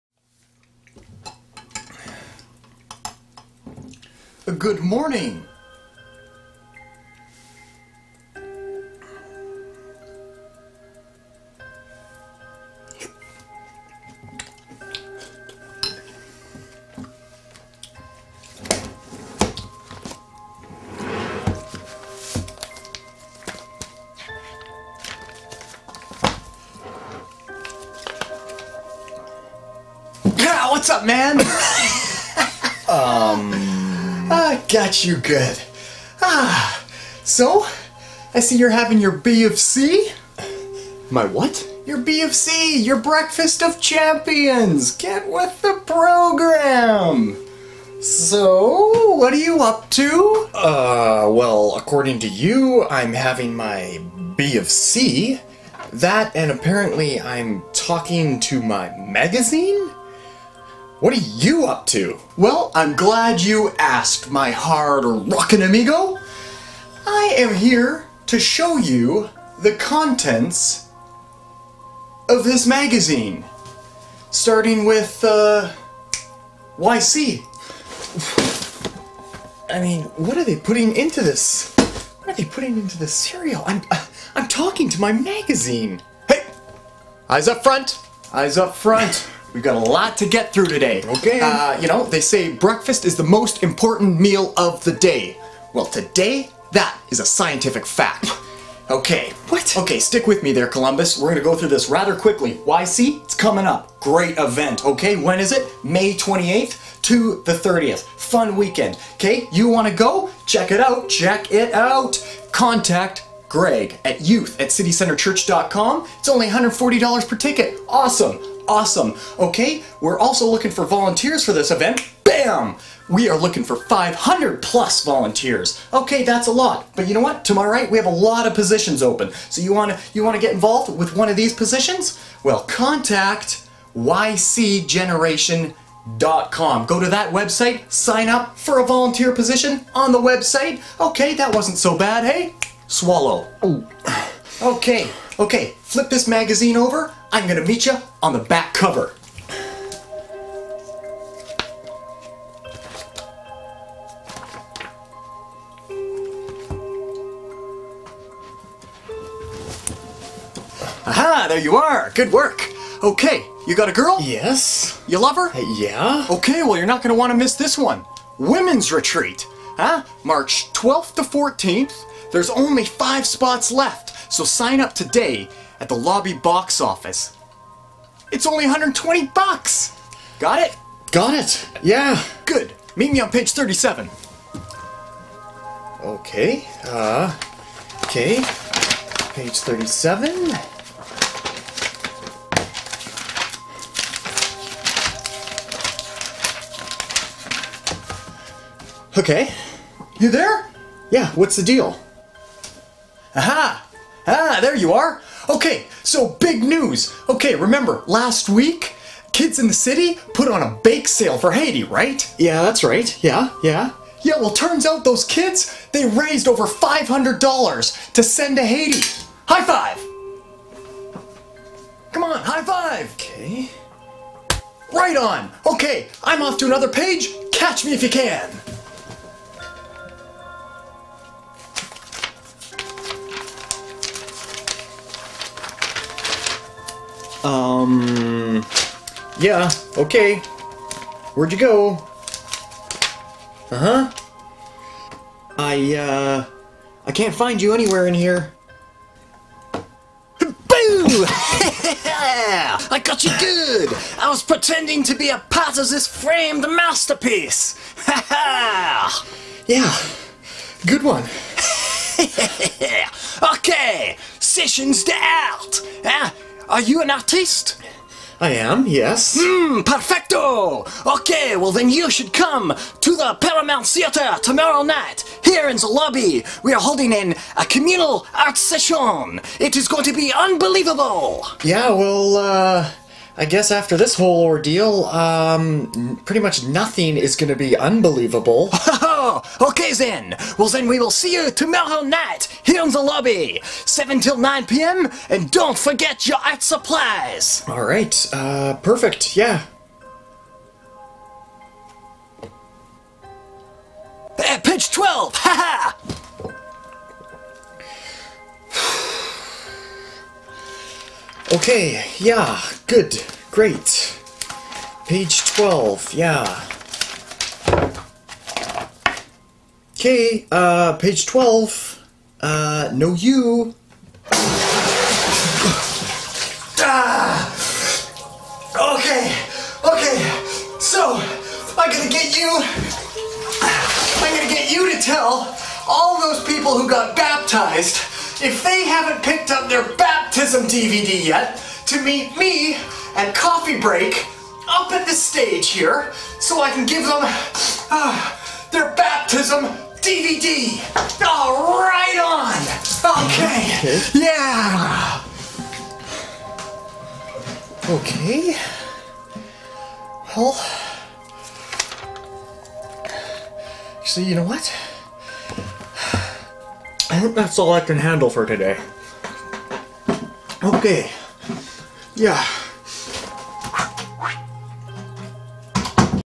City Centre Church : "Magazine" Announcement video
This is an announcement video made for City Centre Church, to promote their upcoming events.